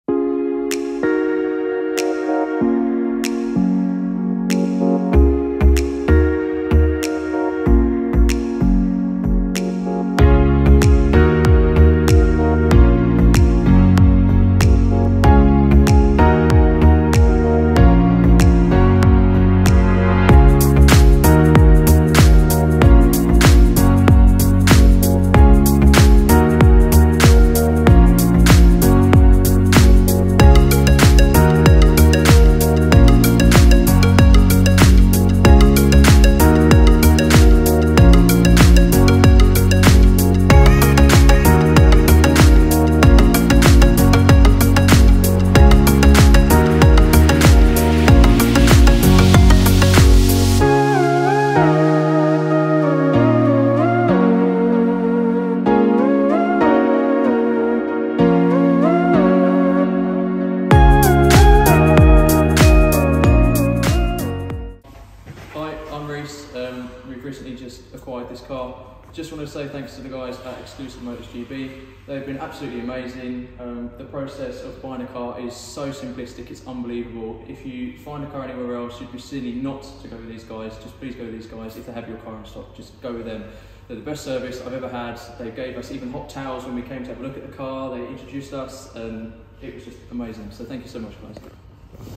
2024 Tesla Model Y RWD - Exclusive Cars (GB) -
We are delighted to present this like new 2024 Tesla Model Y. This Tesla Model Y benefits from the all new and the most expensive paintwork - Cherry Red paint which was a £2,600 option. Configuring a brand new one would cost over £47,500 so therefore, significant savings from new!  The 2024 Model Y also has additional benefits such as the Hardware 4 which includes improvements on the cameras, autopilot functionality and it has more a more refined battery which helps improve the range.  ---|--- Say hello to the Tesla Model Y, which should appeal to buyers looking for extra SUV practicality, with the capability of achieving circa 280 miles driving range on a full charge and ability to achieve a staggering 0-60mph in just 6.6 seconds. ---|---TESLA WARRANTY UNTIL 06/2028 AND BATTERY WARRANTY UNTIL 06/2032---|--- The Model Y has an extensive specification including: autopilot, satellite navigation , surround cameras, panoramic roof, electric tailgate, heated front and rear seats, premium connectivity, premium black leather, premium audio system, supercharger enable, and more.---|--  Inside, the Model Y has the same minimalist interior, focused around a high-res 15-inch touchscreen in the middle of the dashboard. A taller roof means there's more space and headroom inside the Model Y, while its hatchback boot is much more useful for loading luggage or sports equipment.---|---

With a practical and reliable range, fast charging capability, plenty of space and user-friendly onboard technology, the Model Y will appeal to buyers who need to take care of the family or work basics, but who also want the curb appeal and badge kudos that Tesla provides.  We think that owning this car is akin to owning an Apple product, simple yet brilliant in both form and function and seldom out of date thanks to its over the air updates. Yep. Instead of having to go to a dealer or service centre to get your car updated, Tesla pushes updates to its cars over the net like it’s a massive, wheeled smartphone.